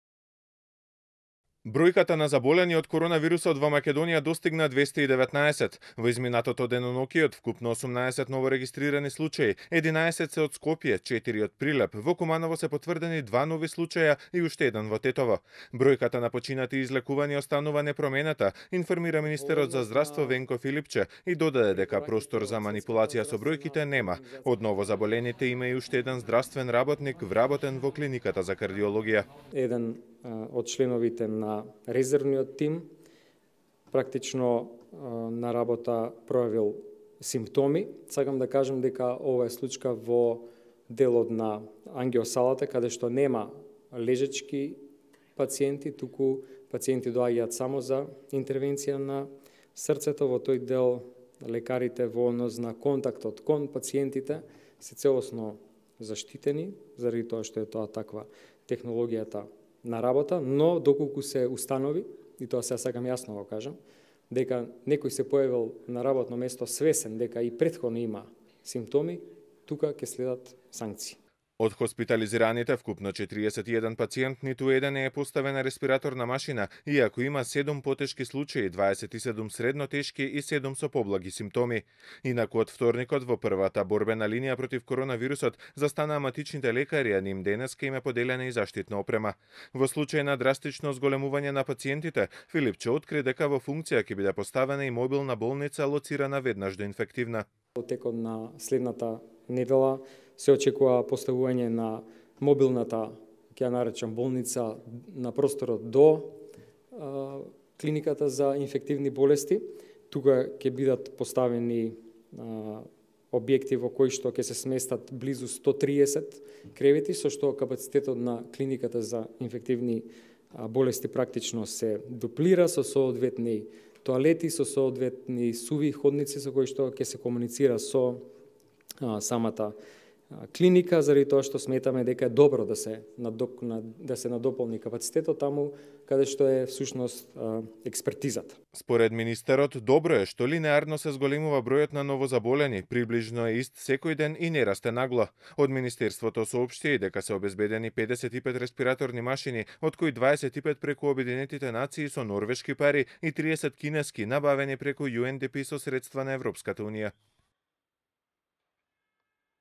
Благ пад на новозаболени од коронавирусот, 18 новоинфицирани, вкупниот број се искачи 219
Бројката на заболени од корона вирусот во Македонија достигна 219. Во изминатото деноноќие, од вкупно 18 новорегистрирани случаи, 11 се од Скопје, четири од Прилеп, во Куманово се потврдени два нови случаја и уште еден во Тетово.